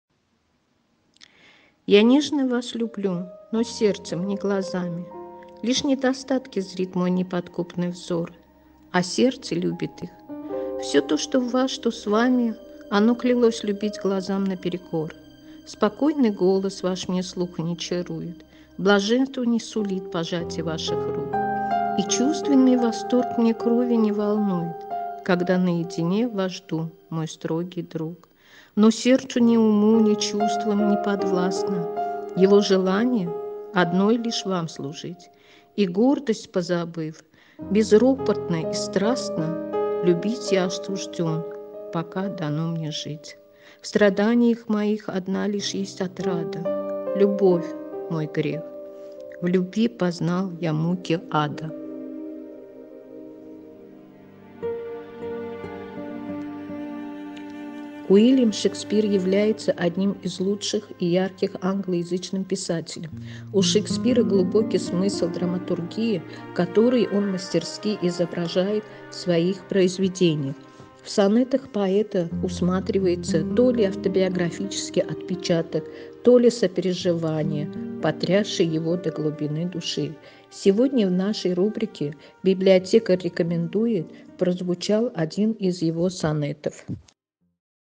У.Шекспира "Сонеты"
26 апреля 1564 года родился Уильям Шекспир , являющийся одним из лучших  и ярких англоязычных писателей.В видеоролике озвучен один из его сонетов, послушайте  его и прочитайте книгу  "Сонеты" У.Шекспира